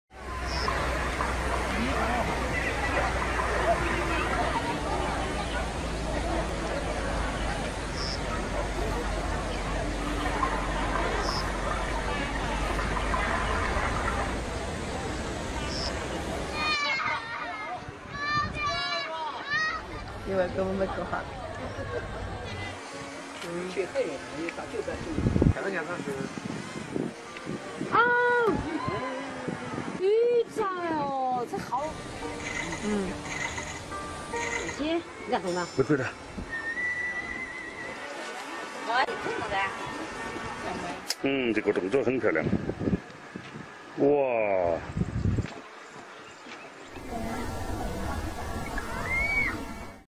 En Chine, les slackliners ont offert une traversée ne tenant qu'à un fil
A Guangzhou, dans le sud-ouest de la Chine, des acrobates d'un autre genre ont offert un spectacle haletant au public présent.…